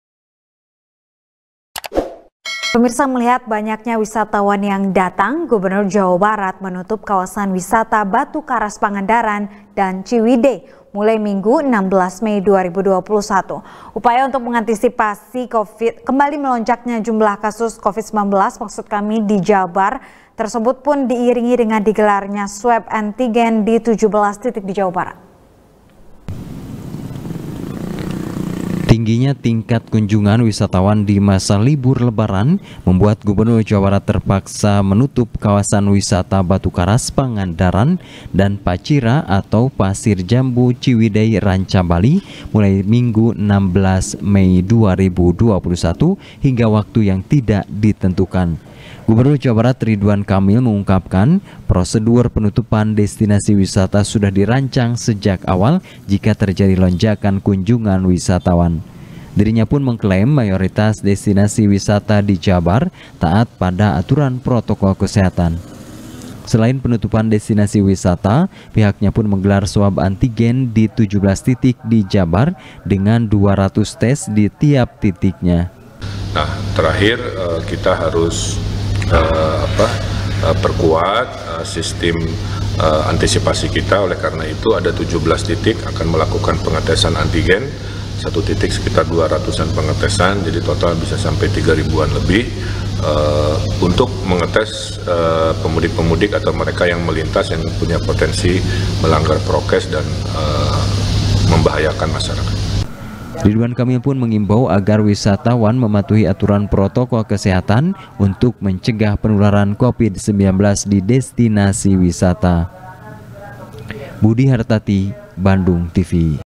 Batu Karas Dan Ciwidey Tutup, Swab Antigen Pun Digelar Di 17 Titik | SBR | BANDUNGTV
SBR | BANDUNGTV | NIKEN PRATIWI | Melihat Banyaknya Wisatawan Yang Datang, Gubernur Jawa Barat Menutup Kawasan Wisata Batu Karas Pangandaran Dan Ciwidey, Mulai Minggu 16 Mei 2021. Upaya Untuk Mengantisipasi Kembali Melonjaknya Jumlah Kasus Covid-19 Di Jabar Tersebut Pun Diiringi Dengan Digelarnya Swab Antigen Di Tujuh Belas Titik Di Jawa Barat.